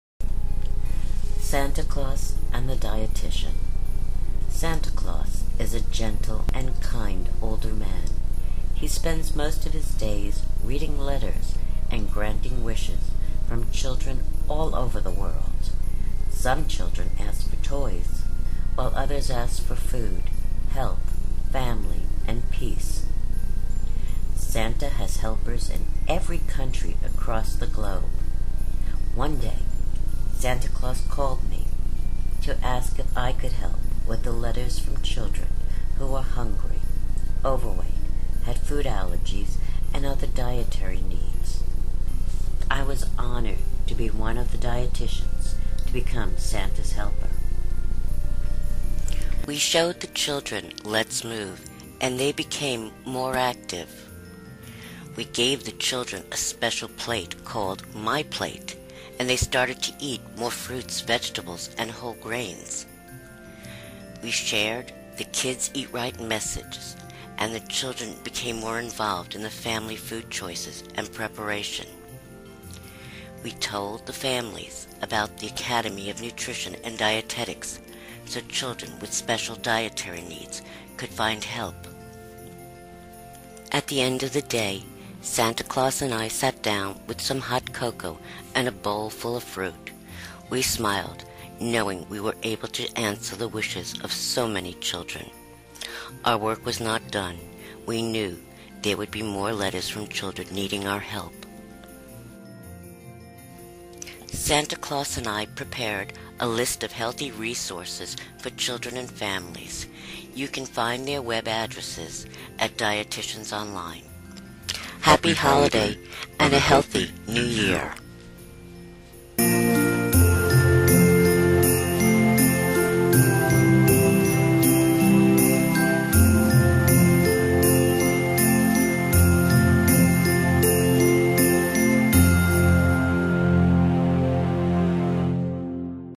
Santa Claus and the Dietitian
Santa Claus is a gentle and kind older man. He spends most of his days reading letters and granting wishes from children all over the world. Some children ask for toys, while others ask for food, health, family and peace. Santa has helpers in every country across the globe. One day Santa Claus called me to ask if I could help with the letters from children who were hungry, overweight, had food allergies and  other dietary needs. I was honored to be one of the dietitians to become Santa's helper.

We showed the children "Let's Move" and they became more active.

We gave the children a special plate, called "MyPlate" and they started to eat more fruits, vegetables, and whole grains.

We shared the "Kids Eat Right" messages and the children became more involved in the family food choices and preparation.

We told the families about the "Academy of Nutrition and Dietetics", so children with special dietary needs could find help.

At the end of the day, Santa Claus and I sat down with some hot cocoa and a bowl full of fruit. We smiled knowing we were able to answer the wishes of so many children. Our work was not done; we knew there would be more letters from children needing our help.

Santa and I prepared a list of Healthy Resources for Children and Families. You can find their web addresses at Dietitians Happy Holiday and a Healthy New Year